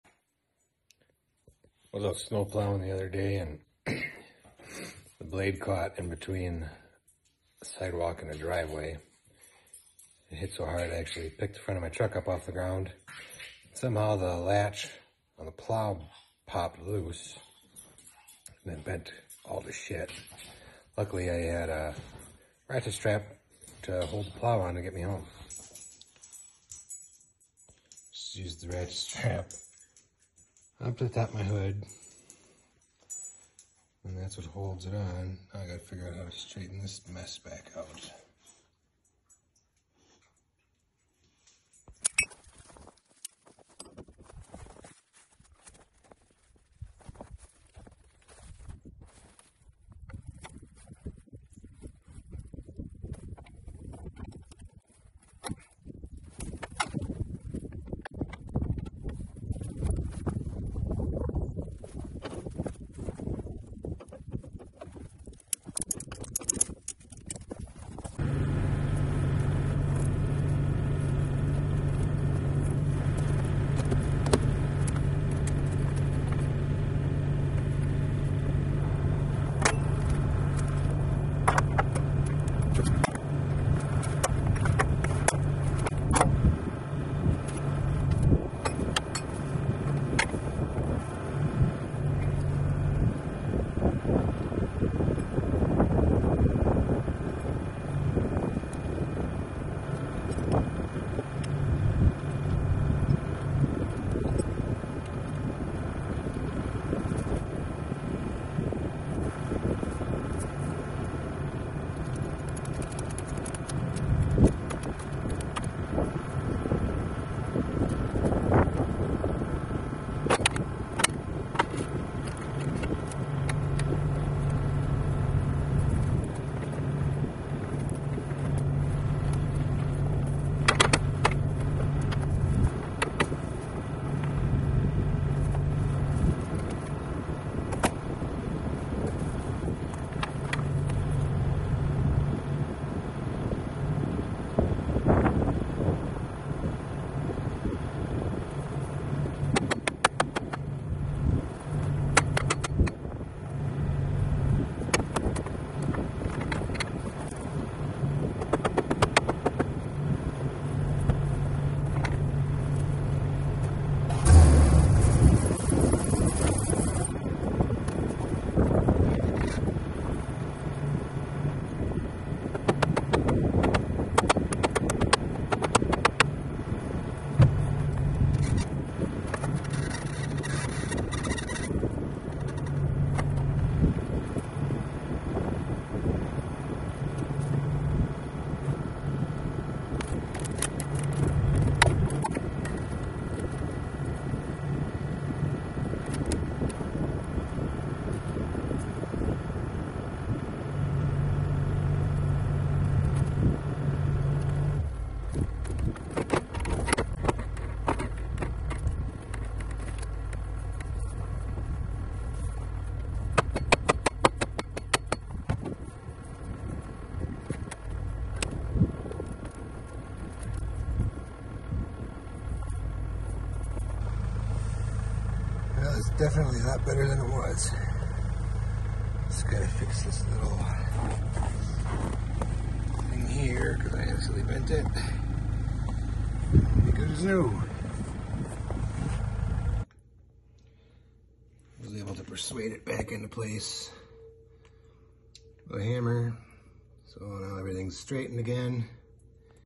Fixing my wester snow plow ultra mount.
In this video I'm straightening the ultra mount, on my western snow plow. It got bent when I was plowing.